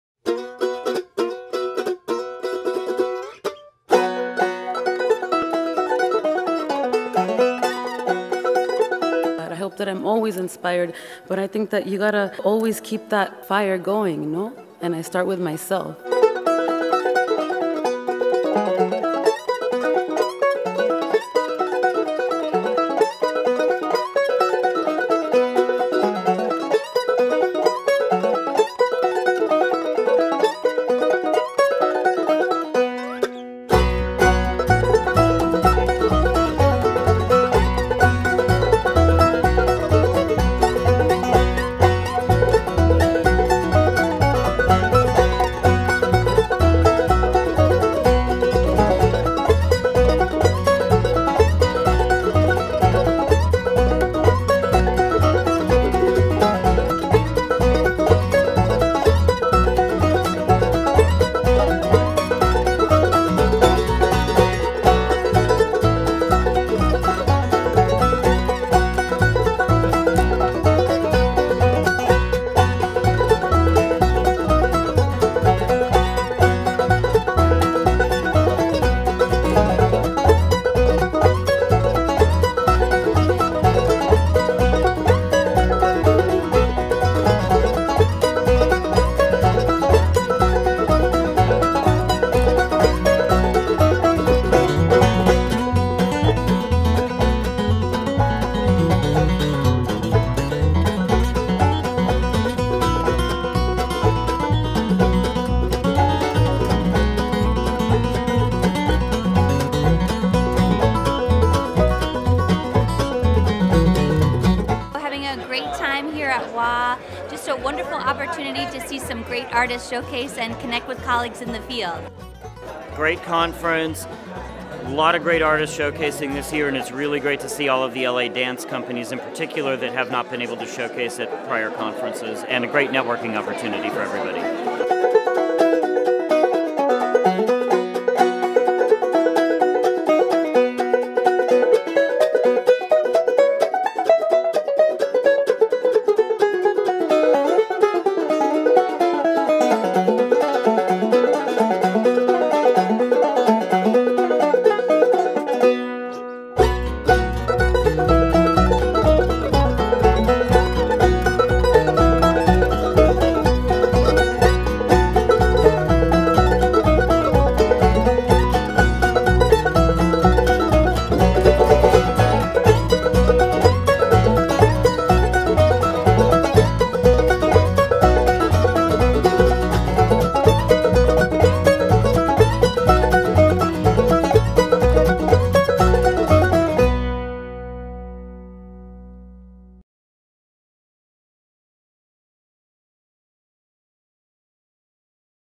2016 Western Arts Alliance Annual Conference Recap
Western Arts Alliance's 2016 conference was its best attended since 2008 and included 190 exhibits, 30 breakouts and sessions, and 200 listed showcases featuring hundreds of performing artists. Thanks to the many committee volunteers, sponsors, funders, speakers, exhibitors, artists, and attendees who contributed to the success of the conference!